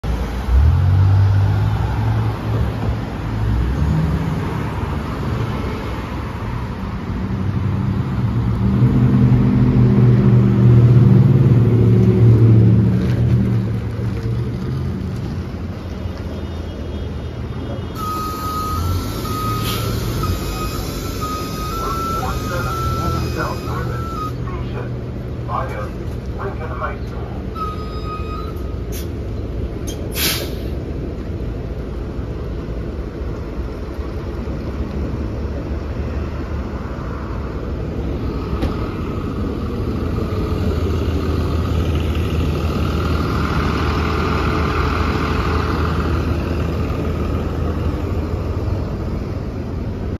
Bee-Line Bus 2009 NABI 40-LFW SCHOOL Tripper Route 470 To MOUNT VERNON STATION #265 YONKERS AVE HEV
ELECTRIC HYBRID BUS FLEET (040.44) VIA LINCOLN HIGH SCHOOL